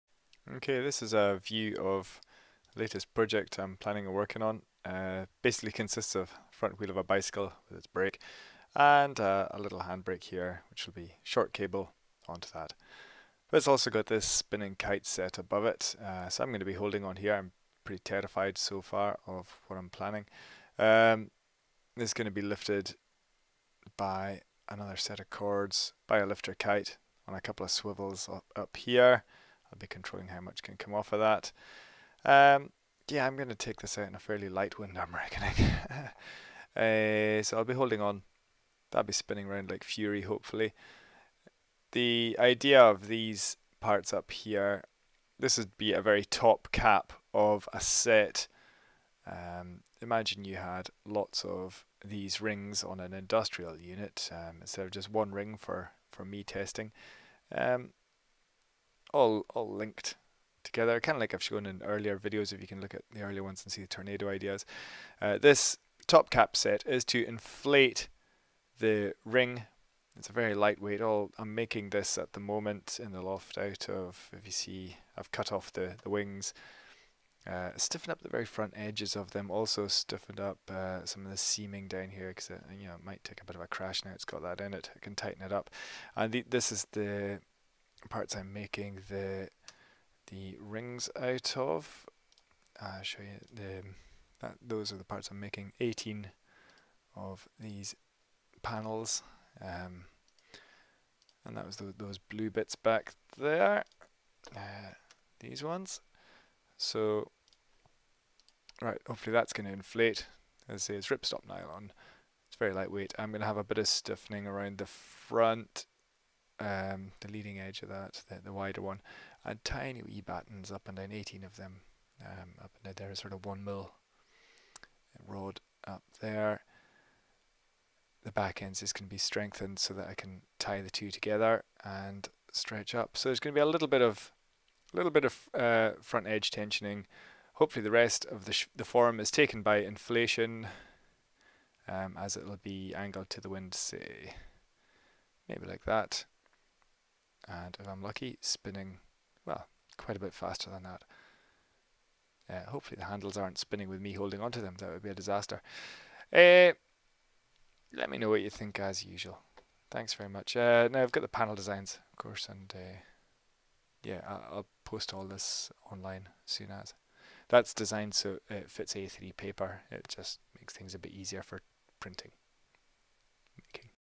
prototyping again
Starting on another spinner make. This time, I'm trying to make a progressively inflating top set.. The top set consists of 3 rings with a slightly stiffened lead edge, and tiny ribs on the panel joins. This way if a ring stays inflated whilst it lifts a larger lower ring... It doesn't collapse. Also stiffening up the ram air leading edges so they don't collapse at low speed before they get going. Cut the ends off the ram air kites to fit to the ring kite part.. This will help keep the leading edge of the ram air open and inflating. Not shown is the lifter kite at the top. open source hardware cc4.0